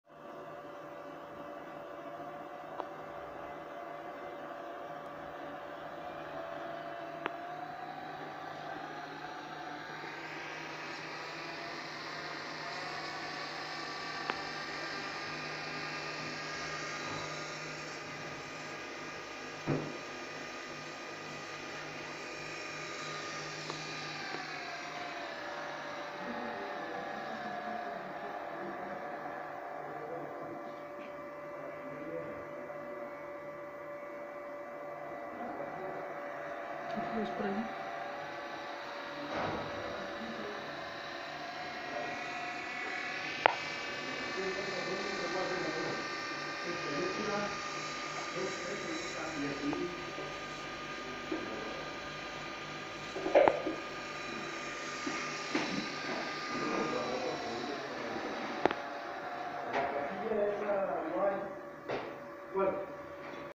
Prueba de servomotor R1511186529
Servomotor
Marca. Fanuc
Modelo. A06B-0373-B175